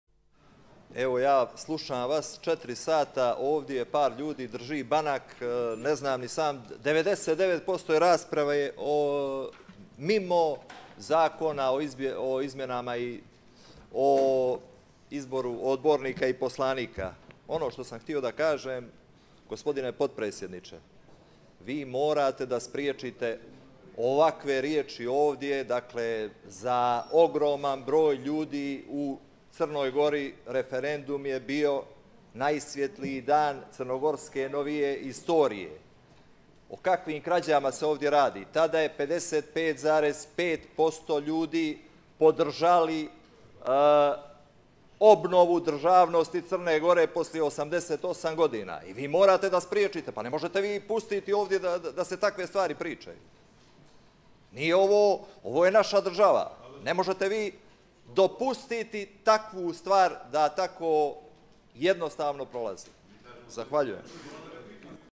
Popović- Rekcija na nipodaštavanje Referenduma iz 2006.
Poslanik Predrag Bulatović nipodaštava je Referendum o nezavisnosti Crne Gore 2006. godine govoreći da je to pokradena izborna volja i namještaljka, na što je reagovao jedino poslanik Liberalne partije Andrija Popović.